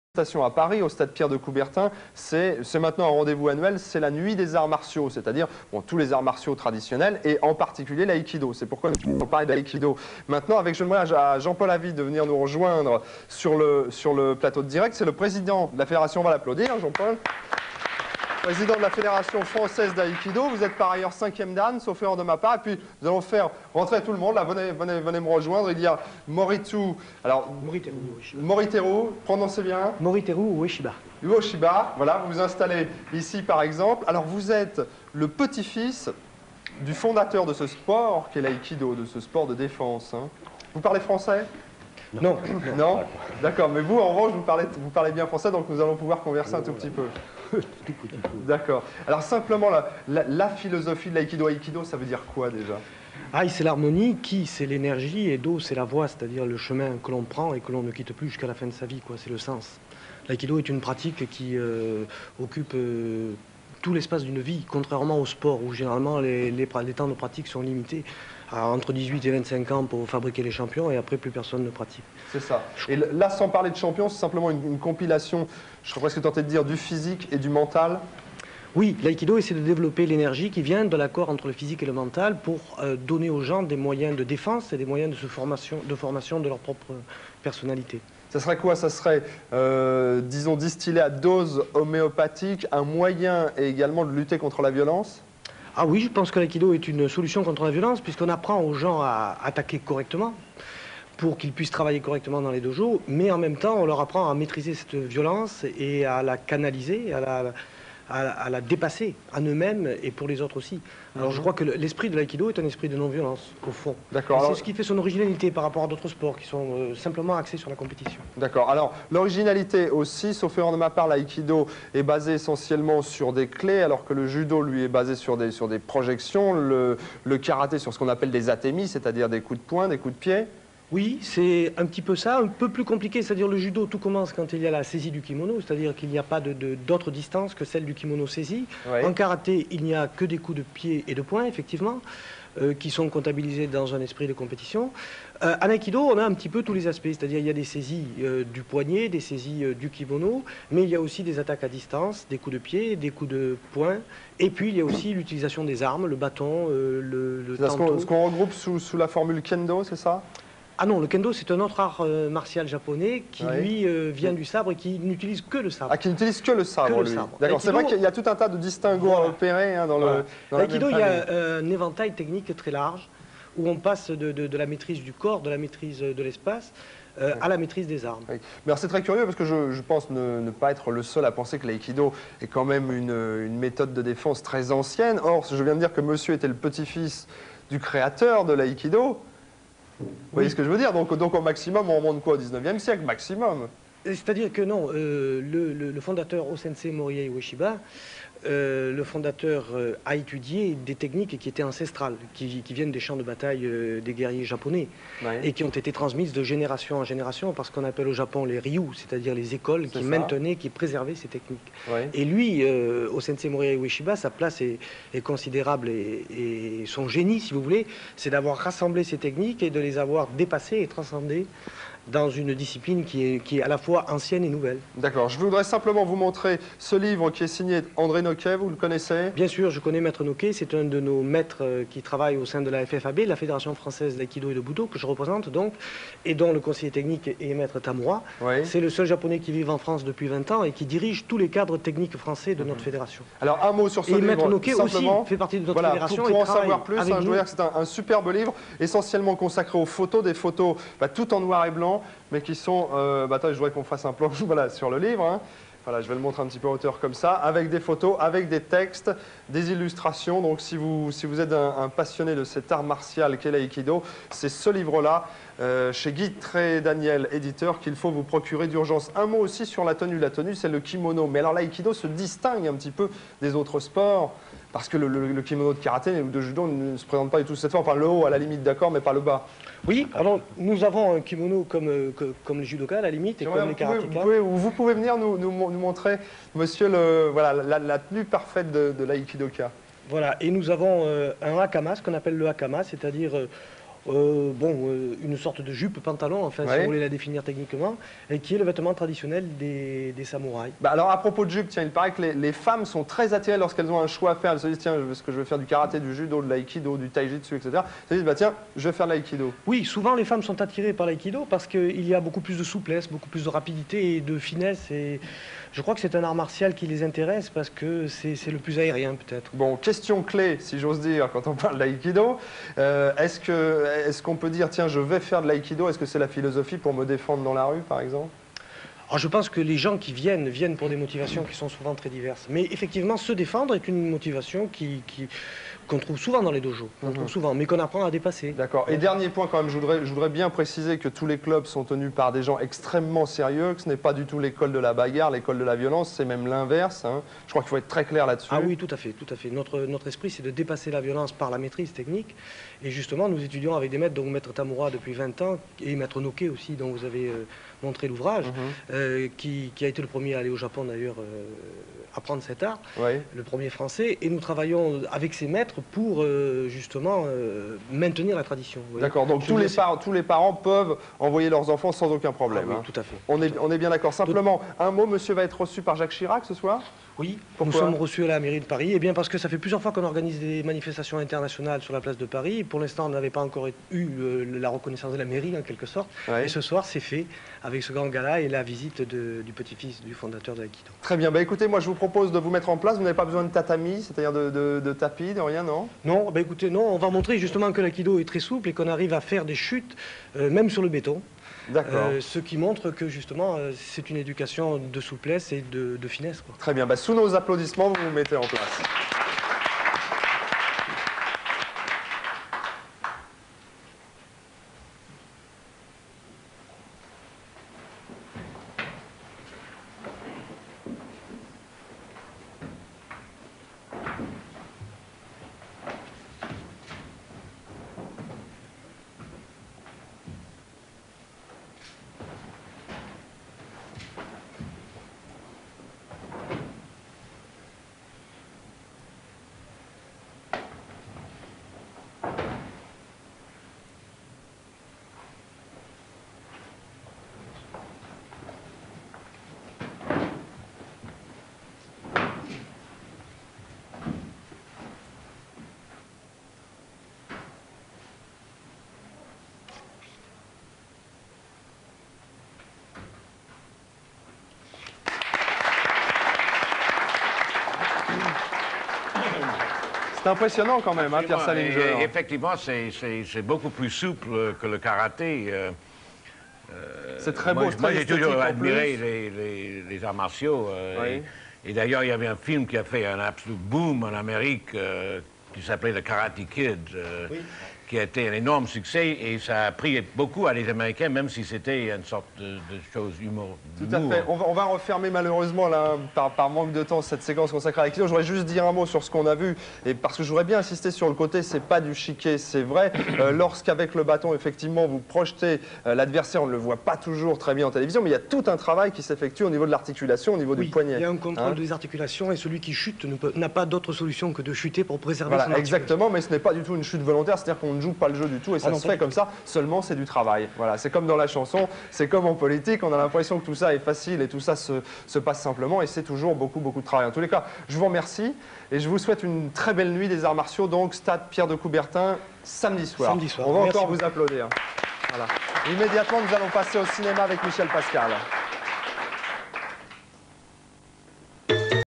Canal Moriteru Tamura Avy
sur Canal+, Ueshiba Moriteru, Tamura Nobuyoshi, Jean-Paul Avy et Jacques Bonnemaison (Daniel Boubo en arriere plan)